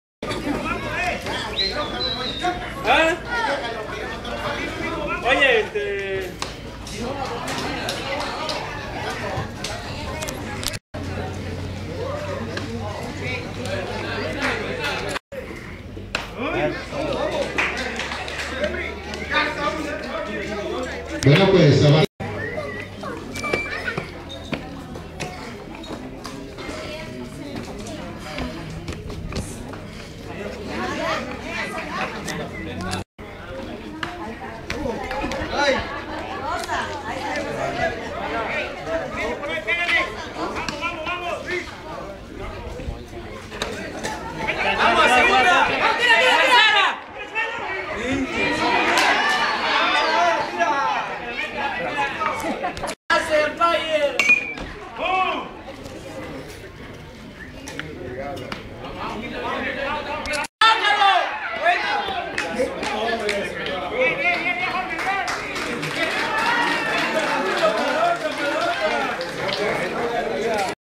AMISTOSO DE BEISBOL INFANTIL 11-12 2
SABADO 22 DE SEPTIEMBRE DEL 2012 EN EL CAMPO DEPORTIVO DE LAS BRISAS EN CHAMPOTON, CAMPECHE ENTRE LOS EQUIPOS ASTROS DE CAMPECHE V.S. GUERRERITOS DE CHAMPOTON.